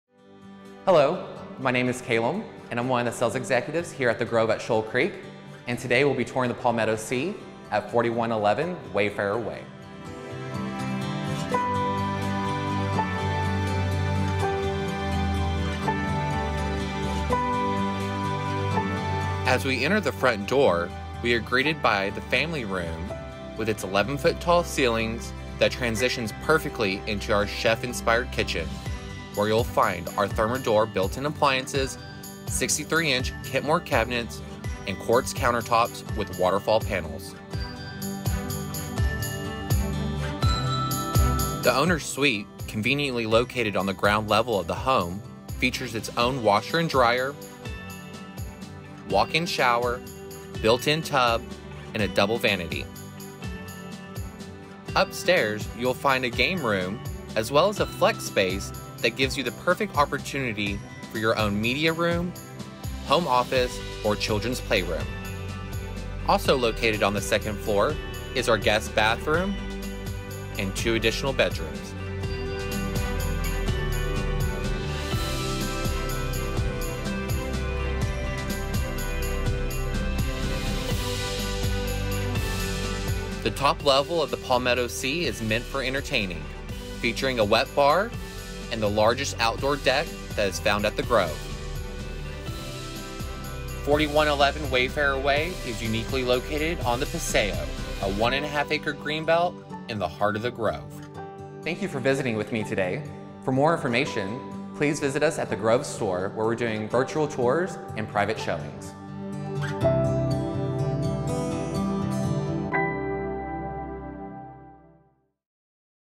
The Palmetto C Floor Plan at 4111 Wayfarer Way
This home on the Paseo offers stunning modern lines and elegant details.  A natural color scheme gives the home a warm and inviting aura. The open concept first floor has an expansive kitchen waiting for a home-chef, or entertainer. Upstairs an impressive second living area and media room await. The third floor wet bar and spacious patio are anticipating their first guests!